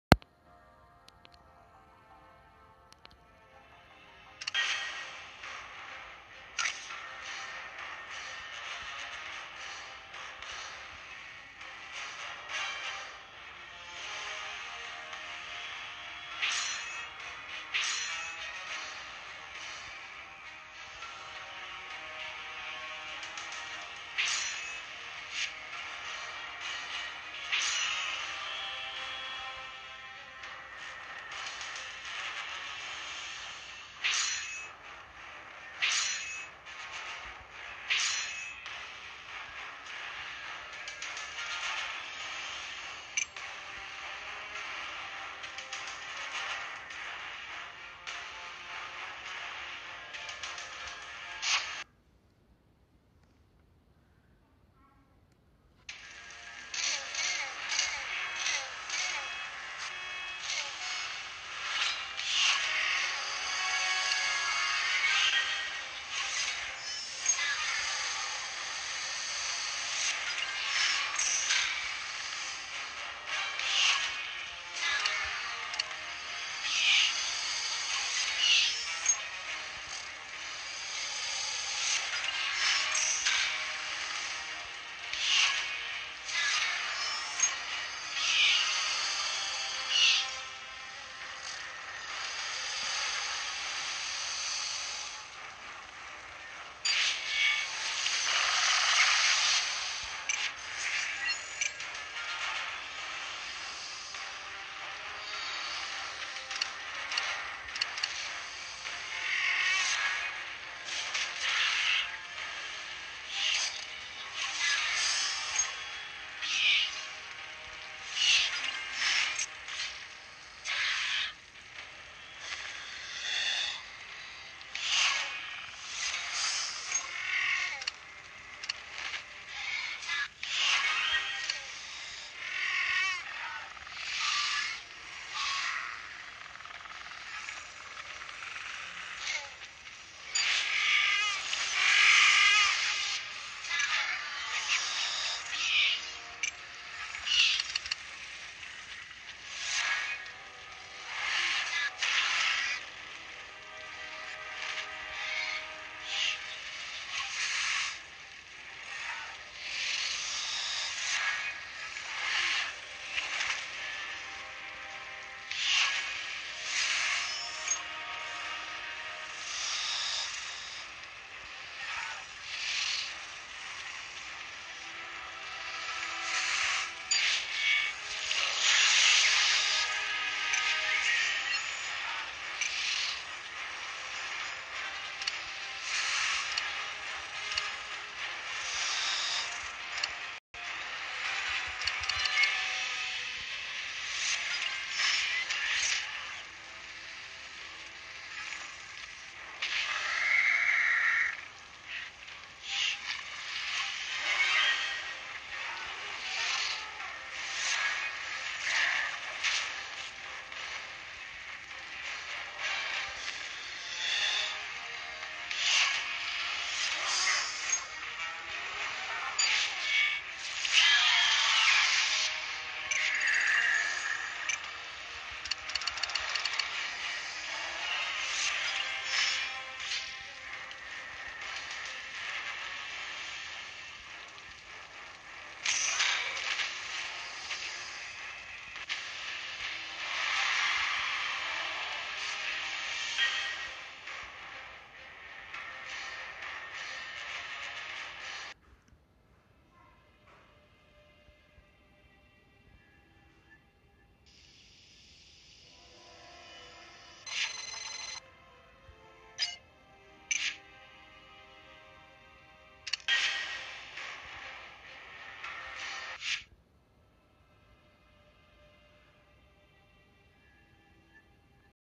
CARNIVORE CARNAGE - BOOSTED - STAGE 1 (2016/11/05)
CARNIVORE CARNAGE - BOOSTED - STAGE 1 (2016/11/05). Beginning to love this BOOSTED battle, everything looks easy! Perhaps next time should give it a try with only 1 DINO!!